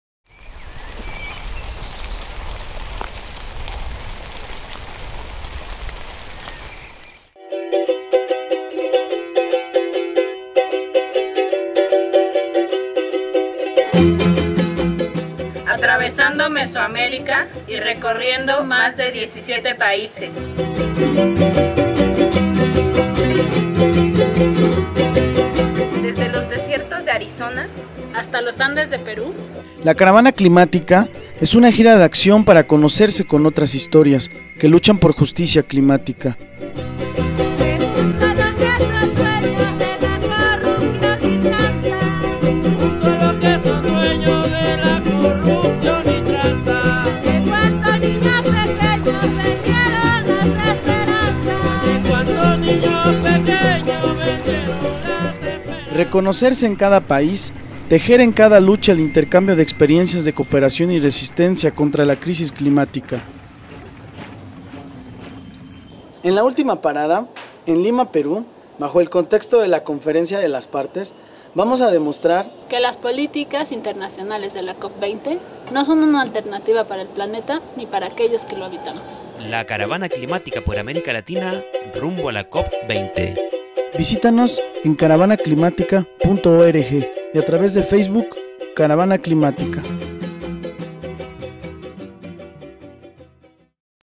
Caravana Climática, síguenos!
Durante 2014, la Caravana Climática por América Latina viajará desde México hasta América del Sur, pasando por 17 países de la región, realizando eventos climáticos públicos, incluyendo exhibiciones multi-medios y de arte, presentaciones y talleres socio-ambientales, y una campaña inspiradora de acciones directas no-violentas y movilizaciones con el objetivo de dirigir la atención y las demandas del público general hacia nuestro destino principal en la ruta de la caravana- la COP20, a realizarse en Lima, Perú, en diciembre. ¡Por favor, contribuyen a nuestros esfuerzos de llegar a América del Sur, y a realizar eventos en el camino!